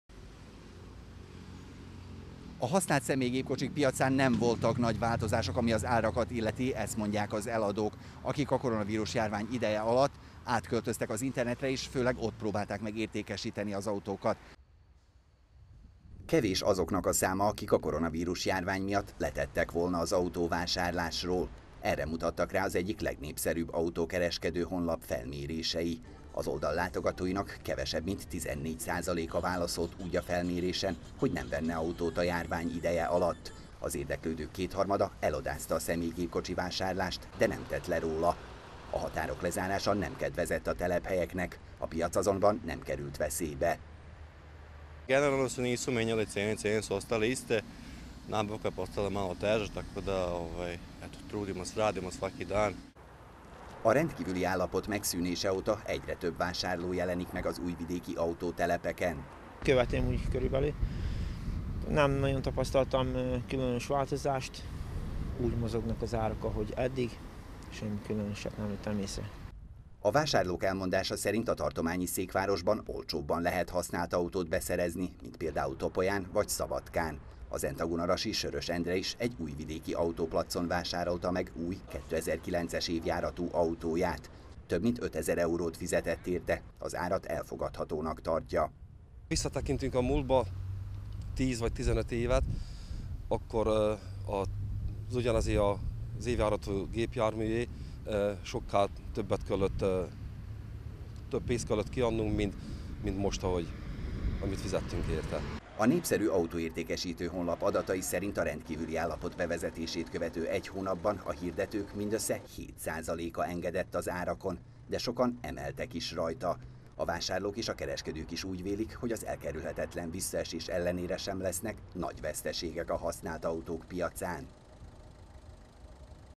2020.05.12. - Nem befolyásolta a használt autók árát a koronavírus-járvány
Március 16-a és április 16-a között 117 ezer hirdetést elemzett ki egy használt autók értékesítésével foglalkozó honlap, és az eredmények szinte semmiben sem különböztek a járvány előtti időszaktól.